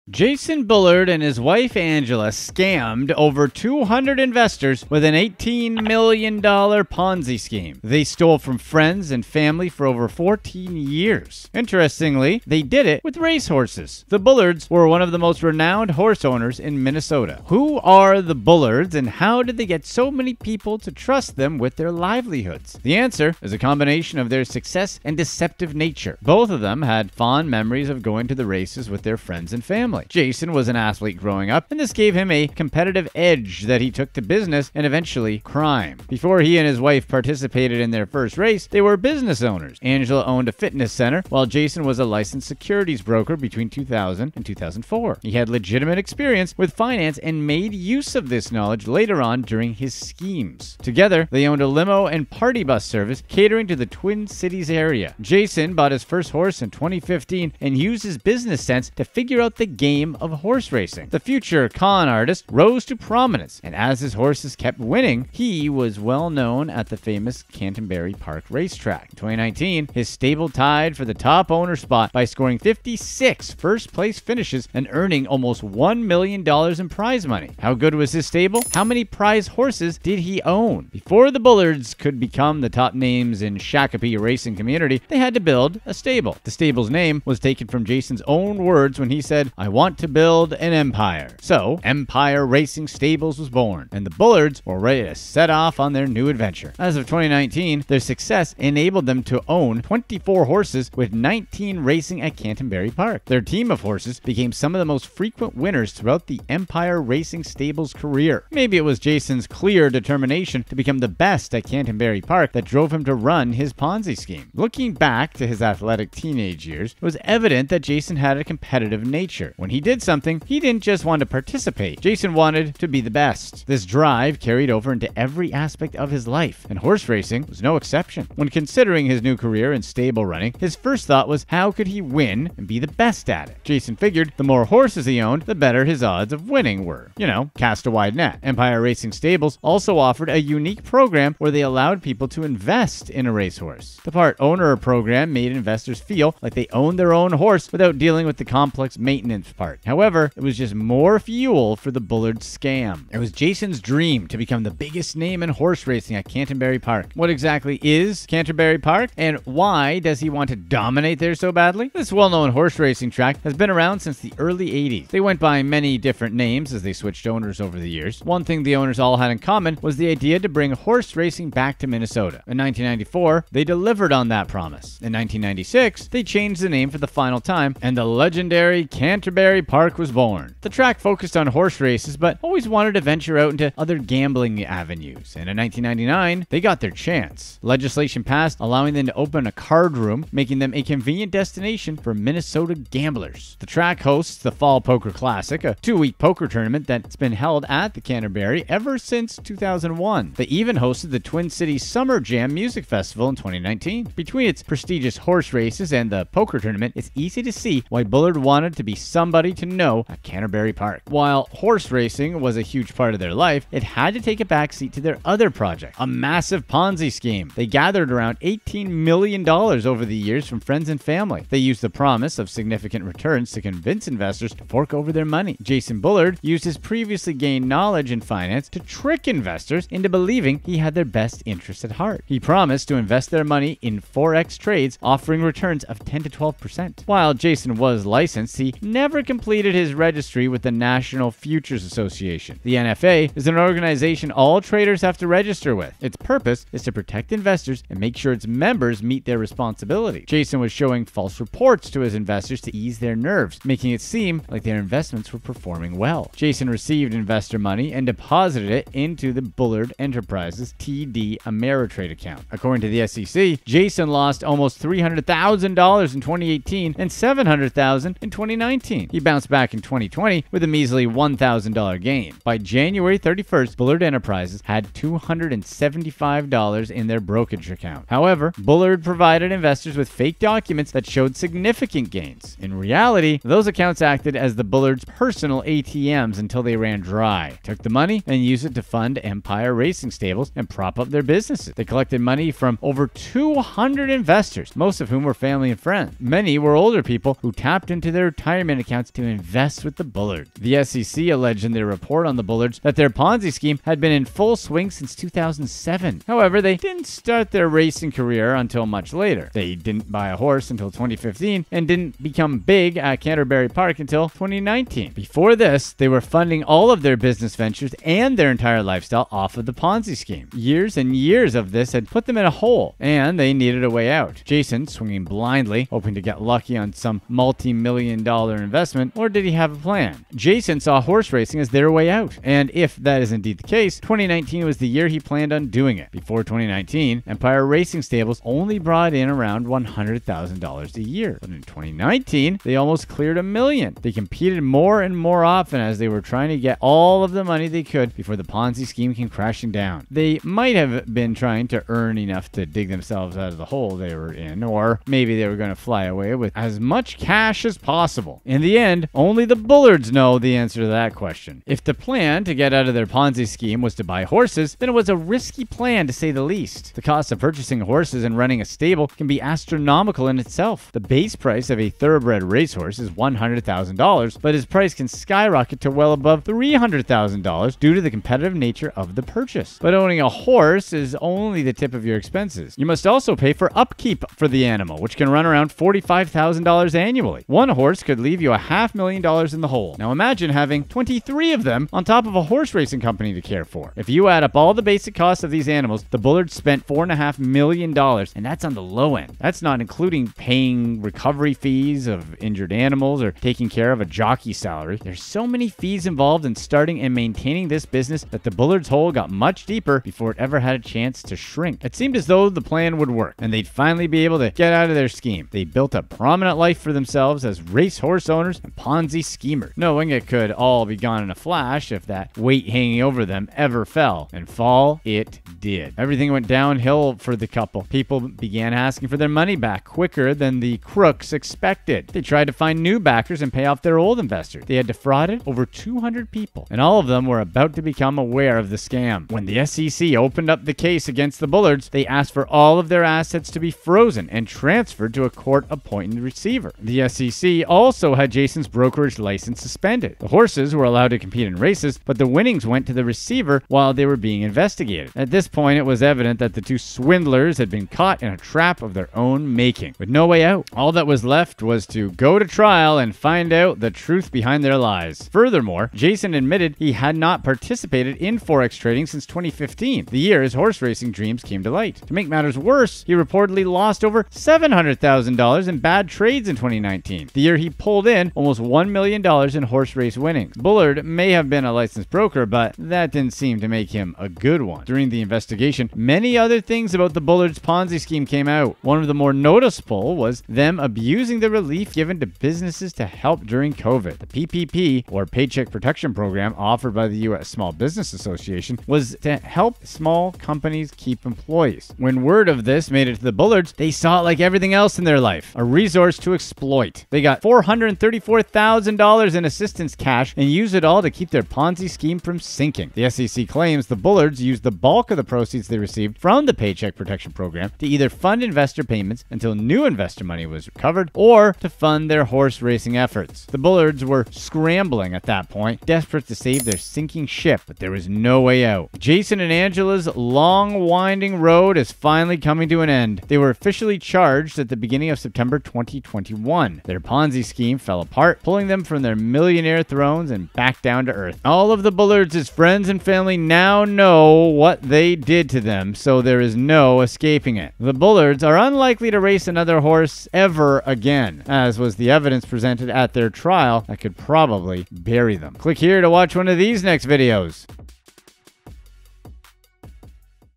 The $18 Million Racehorse Ponzi Scheme
This Minnesota couple dreamt of being famous racehorse owners. Find out why they thought an $18 Million Ponzi scheme was their ticket to fame.

A Winner’s Attitude
Jason Bullard wanted to be a winner. He grew up admiring horses and playing sports, always wanting to be the best at whatever he touched. However, Jason was a huge loser. Unfortunately, for over 200 investors, that led him and his wife, Angela, to running an $18 Million Ponzi scheme.

Empire Racing Stables
Before the Bullards could become the top names in the Shakopee racing community, they had to build a stable. The stable's name was taken from Jason's own words when he said, "I want to build an empire."

Canterbury Park
It was Jason's dream to become the biggest name in horse racing at Canterbury Park. What exactly is Canterbury Park, and why does he want to dominate there so badly? This well-known horse racing track has been around since the early '80s.

The Scam
While horse racing was a huge part of their life, it had to take a backseat to their other project, a massive Ponzi Scheme. They gathered around $18 million over the years from friends and family.

The SEC alleged in their report on the Bullards that their Ponzi Scheme had been in full swing since 2007. However, they didn't start their racing career until much later. They didn't buy a horse until 2015 and didn't become big at Canterbury park until 2019.

Expensive Horses
If the plan to get out of their Ponzi Scheme was to buy horses, then it was a risky plan, to say the least. The cost of purchasing horses and running a stable can be astronomical in itself.

Did Not Finish
It seemed as though the plan would work, and they'd finally be able to get out of their scheme. They'd built a prominent life for themselves as racehorse owners and Ponzi schemers, knowing it could all be gone in a flash if that weight hanging over them ever fell—and fall it did.

PPP Fraud
During the investigation, many other things about the Bullards Ponzi Scheme came out. One of the more noticeable was them abusing the relief given to businesses to help during Covid.

A Long Race Ahead
Jason and Angela's long winding road is finally coming to an end. They were officially charged at the beginning of September 2021. Their Ponzi scheme fell apart, pulling them from their millionaire thrones and back down to earth.